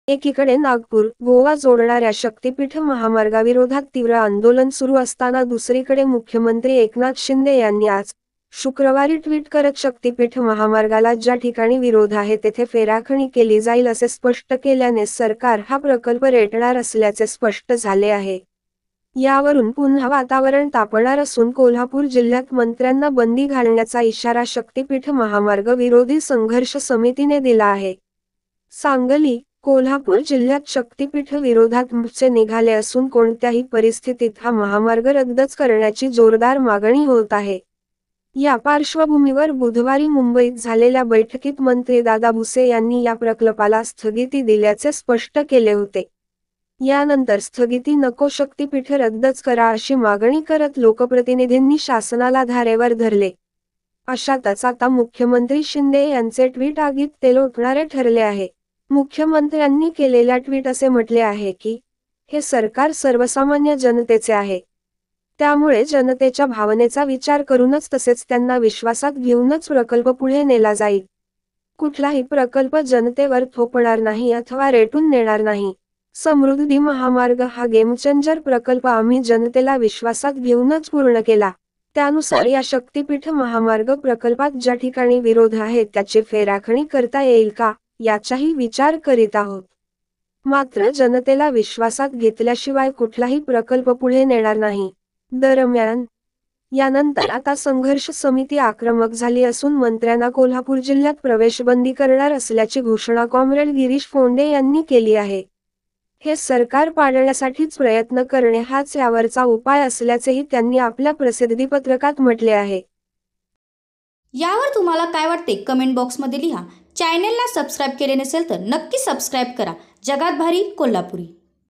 शक्तिपीठ महामार्ग आंदोलन पेटणार..? : शासन प्रकल्प रेटणार: Kolhapur News
शक्तिपीठ महामार्ग आंदोलन पेटणार..? : शासन प्रकल्प रेटणार
: Kolhapur News